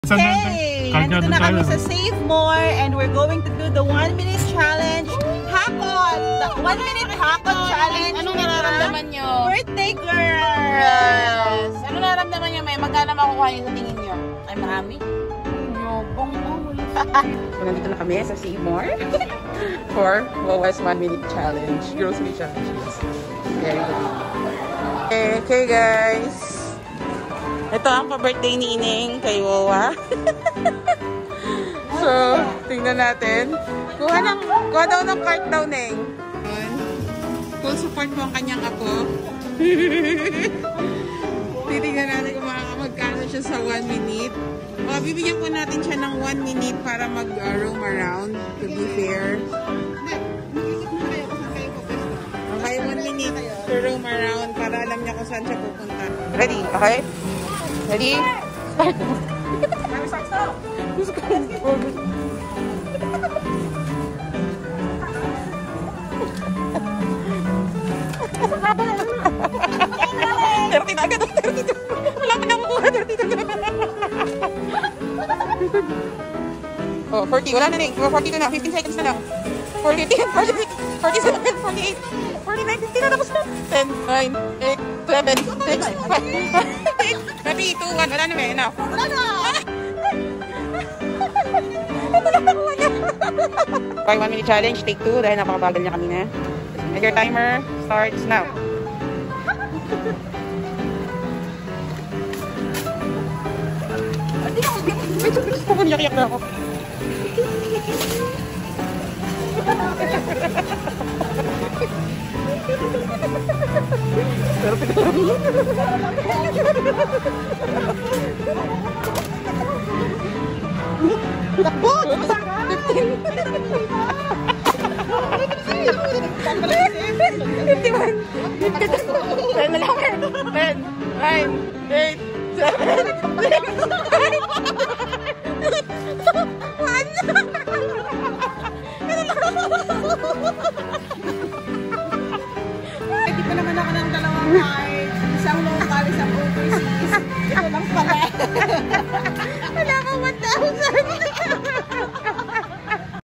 My Wowa’s 1 Minute Grocery “Hakot” Challenge!
It’s my Wowa’s 66th Birthday today and my TitaNi decided to throw a Hakot Challenge for her today🤣 How much do you think she will get?🤩